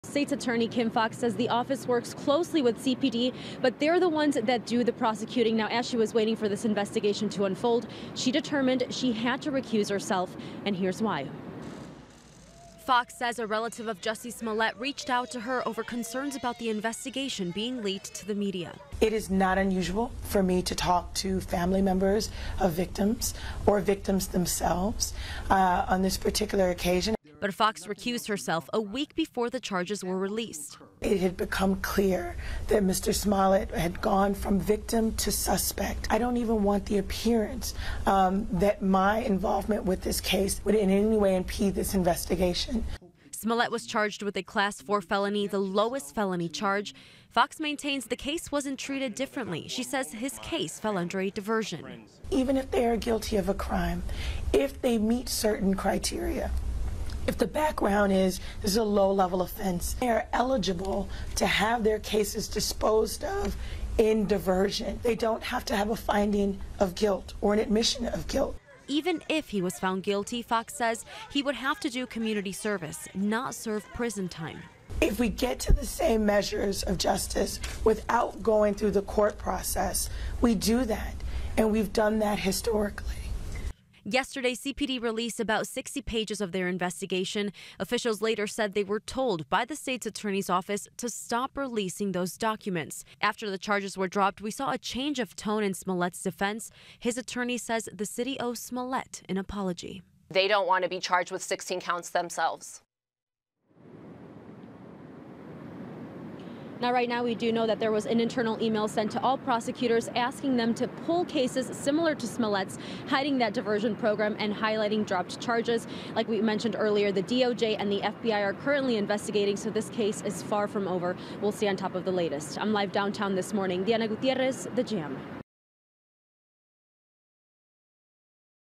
State’s Attorney Kim Foxx Speaks Out On Smollett Case
Chicago is in turmoil after all the charges against Jussie Smollett were dropped by State’s Attorney Kim Foxx's team of prosecutors.

Foxx says the office works closely with CPD and they do the prosecuting. As the investigation unfolded, she determined she needed to recuse herself.

A relative of Smollett reached out to Foxx over concerns about the investigation being leaked to the media, back when Smollett was still considered victim.
Foxx adds Smollett’s case wasn’t treated differently. Smollett was charged with a class four felony, the lowest felony charge making it a possibility for the case to fall under a diversion.
“…even if they’re guilty of a crime, if they meet certain criteria, if the background is a low level offense…,” says Foxx. “They’re eligible to have their case disposed of in diversion… they don’t have to have a finding of guilt or admission.”
Even if he was found guilty Foxx says he would have to do community service, not serve prison time.
After CPD released about 60 pages of their investigation reports, officials said they were told by the state’s attorney office to stop giving away those documents.
After charges were dropped there was a change of tone in Smollett’s defense. His attorney says the city owes Smollett an apology.